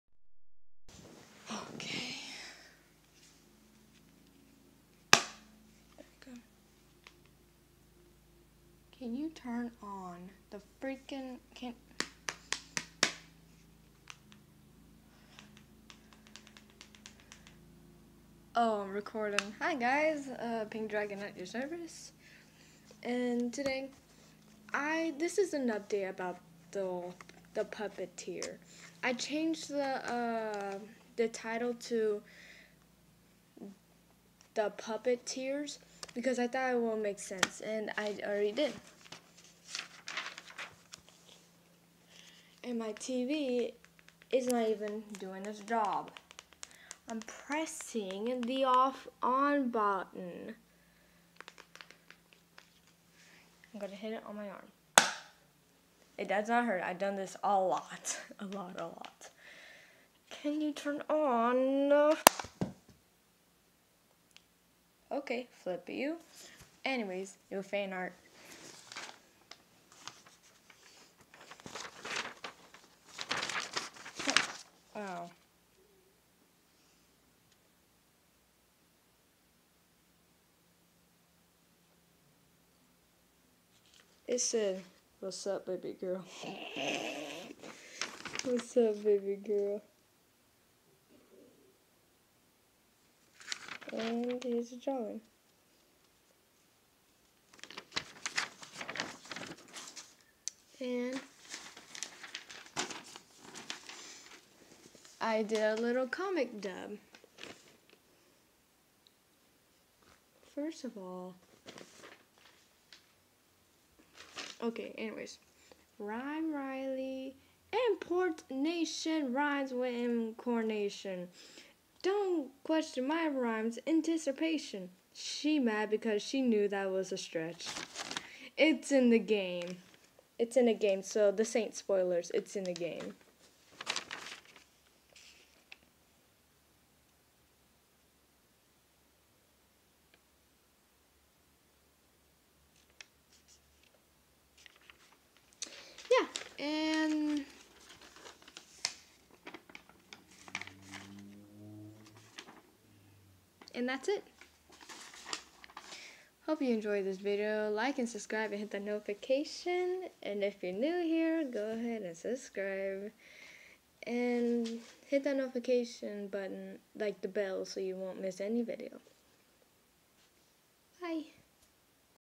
the update on the book and the drawings
the update on my book what in ganna do imma scrap and start all over and work on the docks wile writing it on paper so I can be ready to present my book